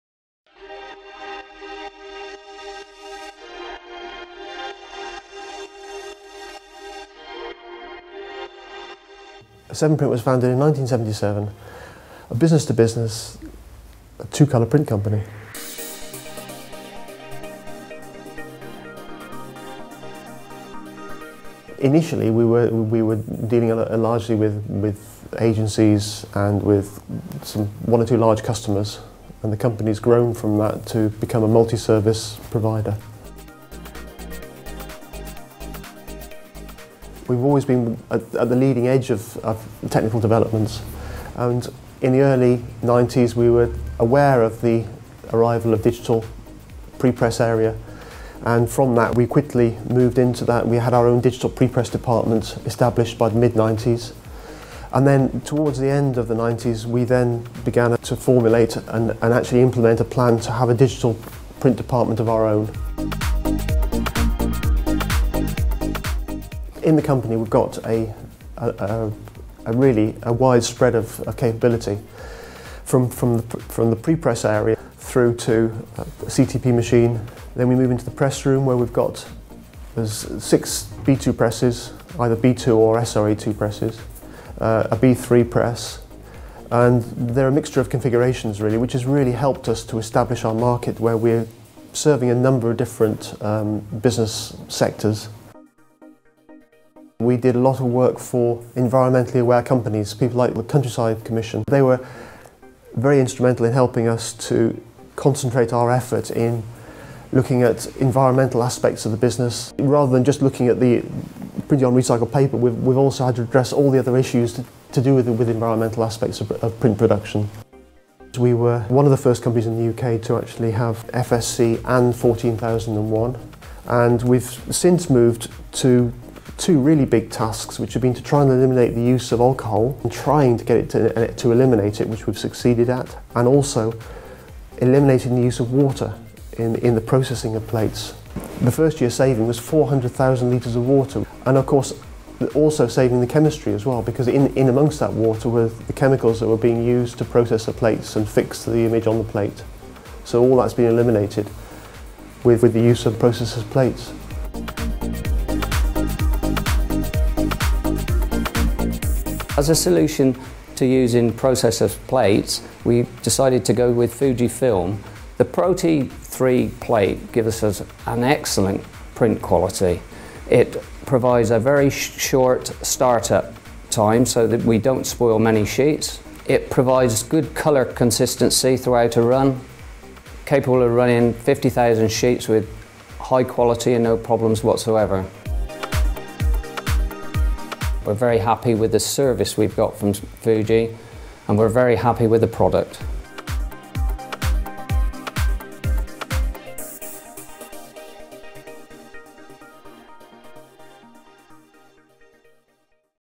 Severnprint reaffirms its environmental credentials with Fujifilm
Gloucester based Severnprint was founded in 1977.  Since then, its founders have been particularly interested in the environment and how the printing processes employed at the company can work as harmoniously as possible with nature.  This short film takes a look at how Severnprint has dramatically reduced water and chemistry usage by choosing to use the Brillia HD PRO-T3 plate from Fujifilm.